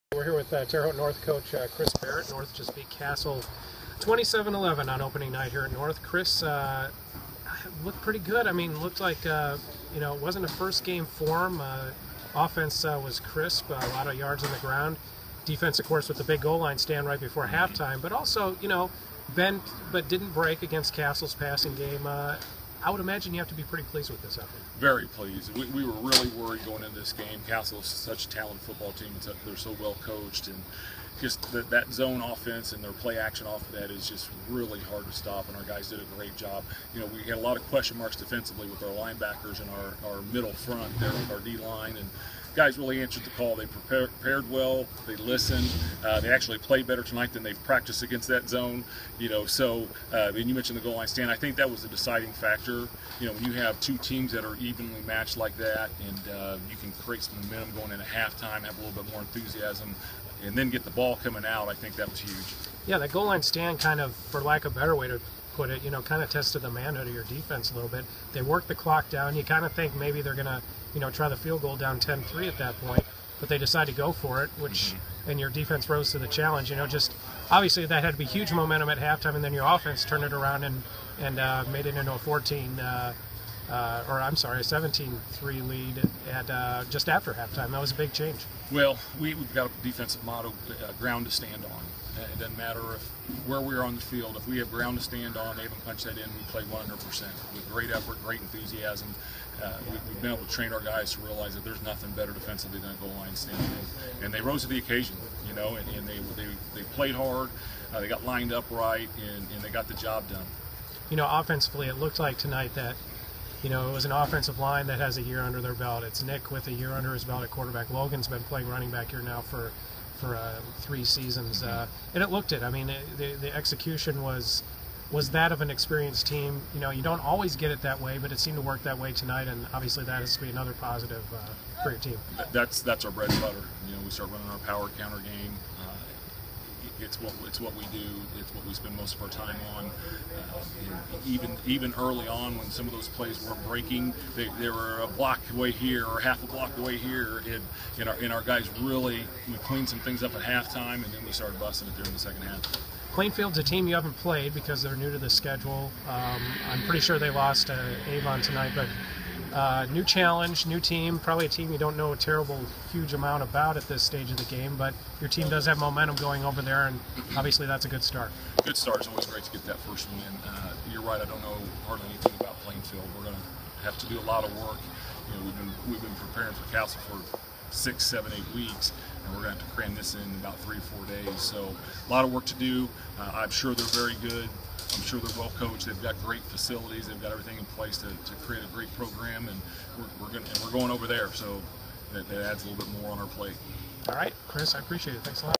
TH North 27, Castle 11 - THN coach Chris Barrett
Chris Barrett talks about North's win over Castle. Recorded on Aug. 21, 2015.